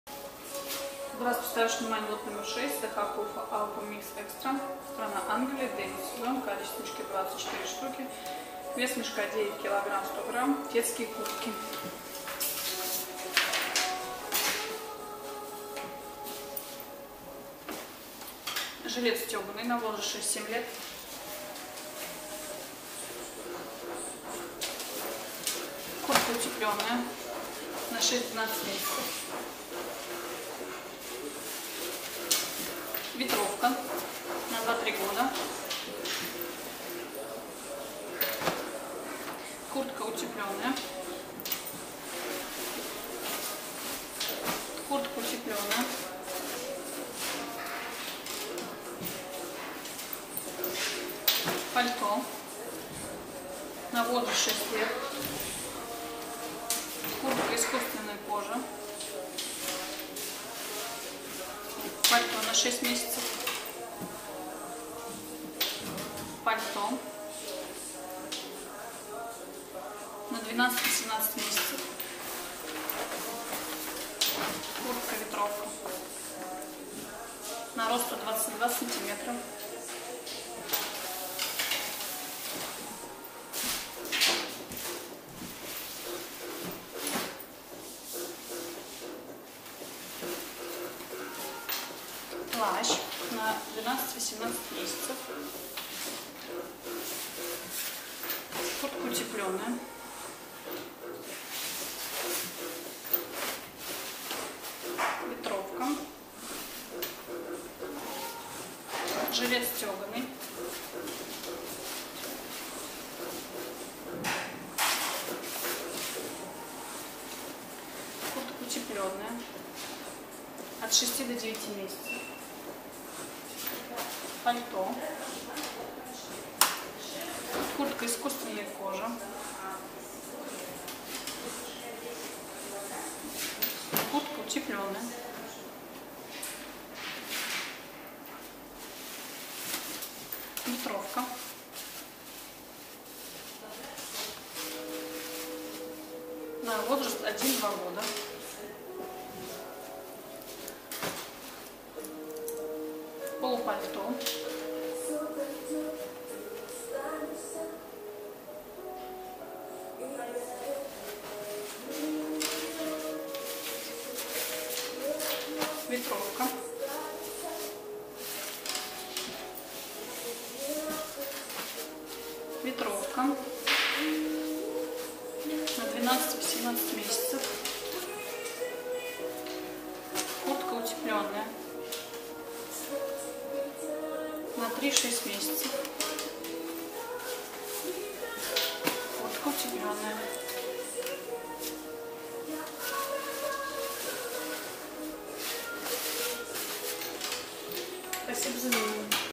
CH PUFFA autumn mix EXTRA 6,секонд хенд одежда оптом
CH PUFFA autumn mix EXTRA (Лот№6)
Детские куртки (секонд хенд)
Страна: Англия
Сезон: Осень
Цена за кг.: 10.53 евро
Вес мешка, кг.: 9.1
Цена за мешок : 95.82 евро